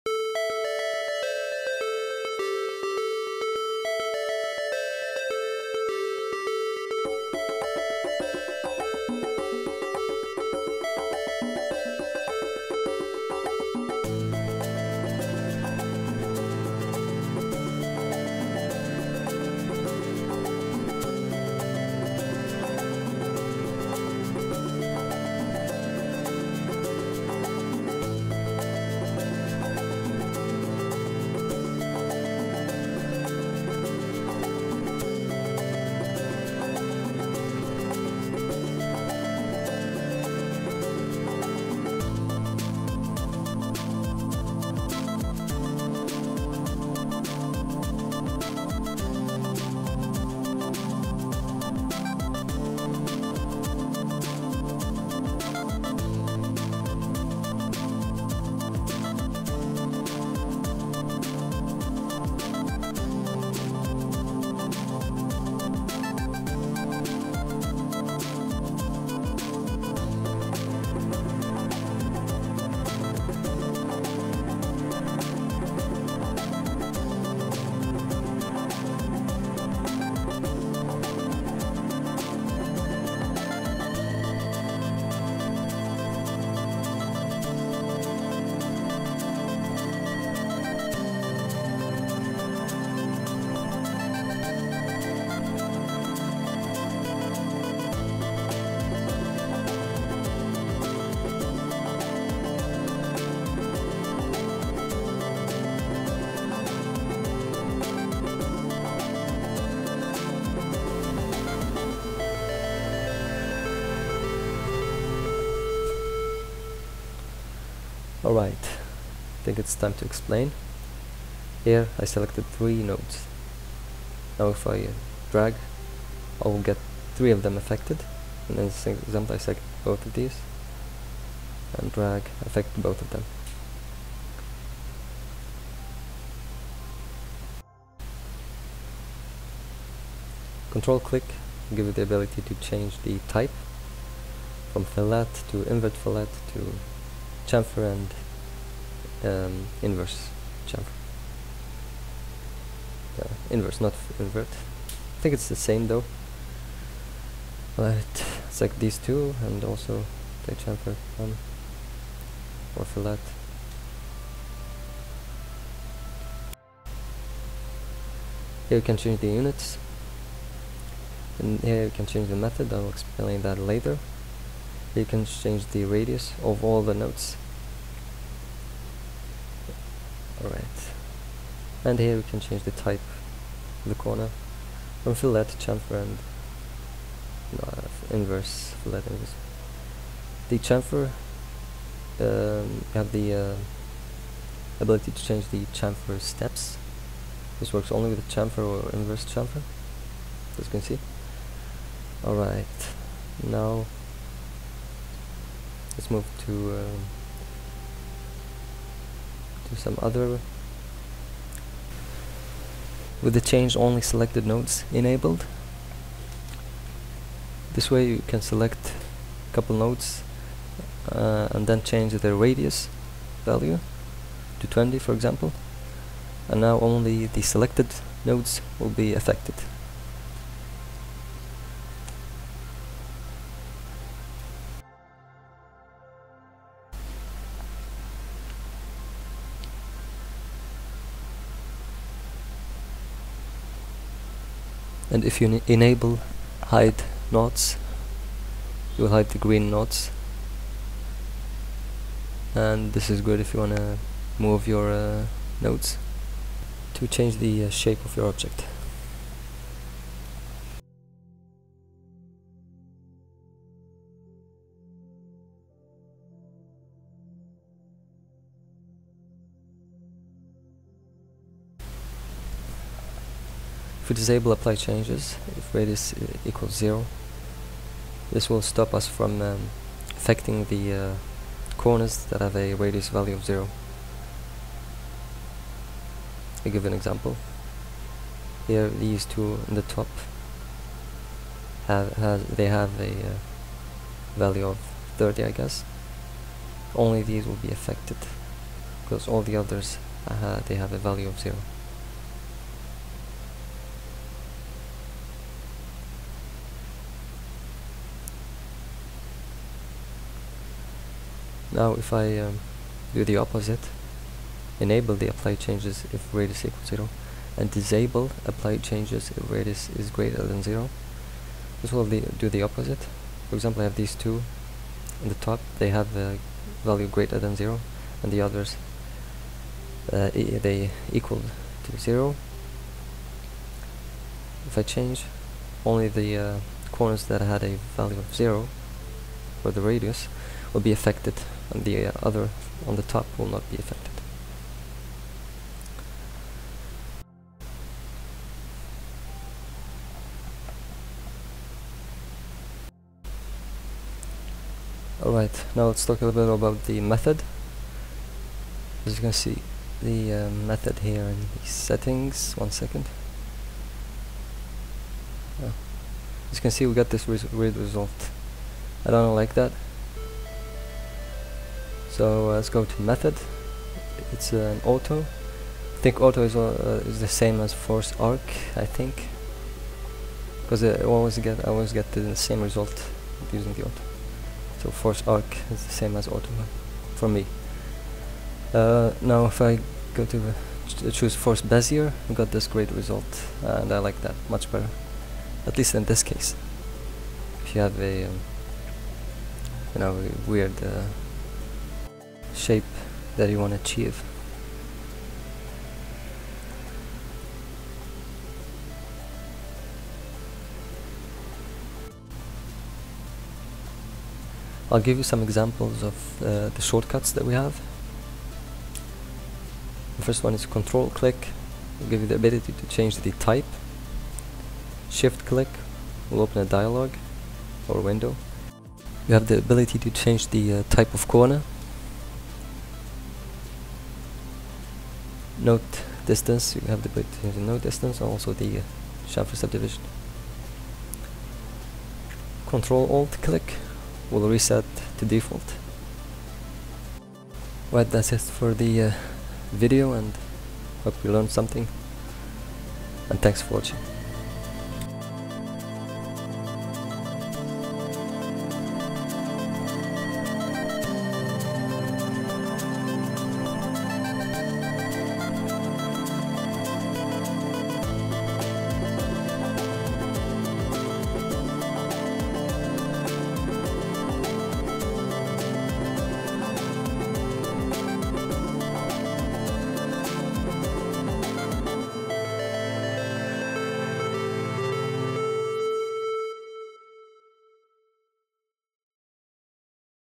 Corners(Fillet/Chamfer) : Inkscape Live Path Effects Tutorial ( LPE )
Another Live Path Effects tutorial, In this one I will cover corners (Fillet/Chamfer).